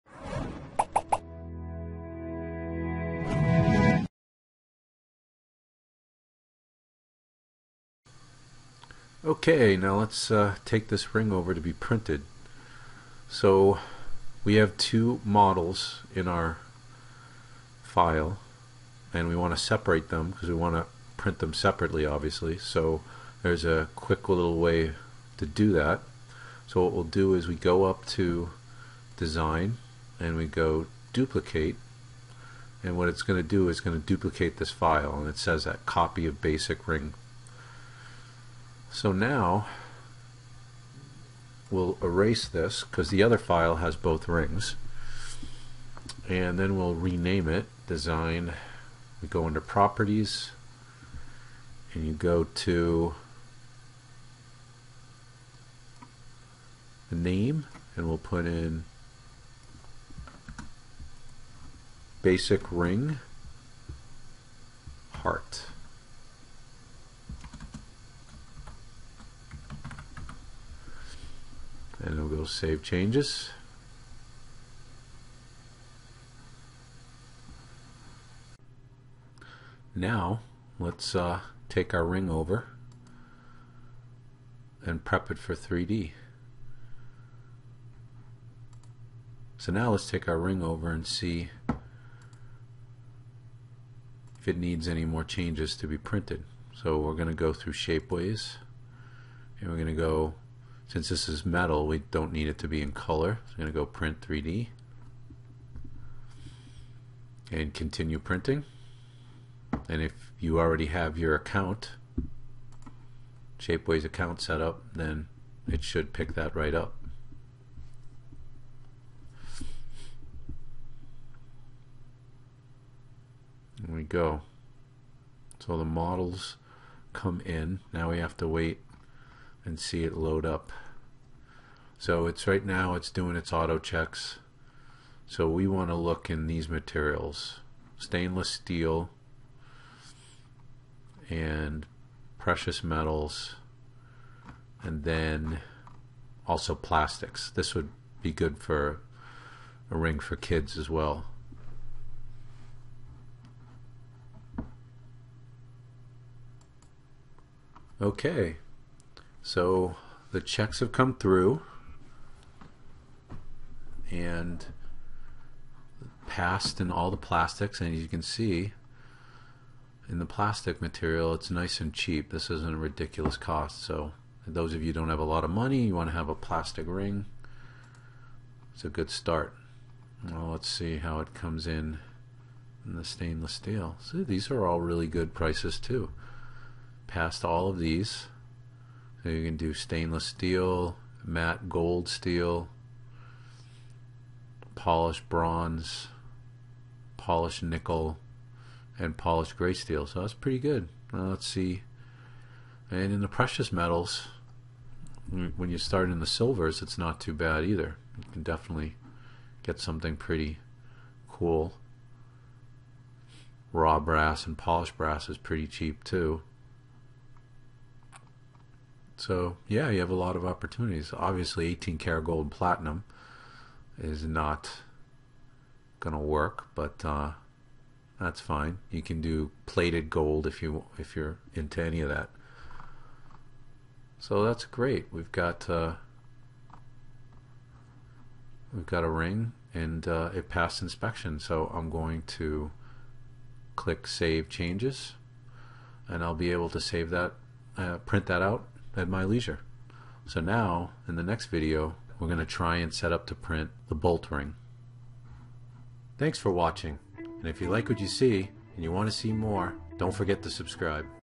How to model and 3d print a Ring pt5
Now we will take our first ring over to Shapeways to be 3d printed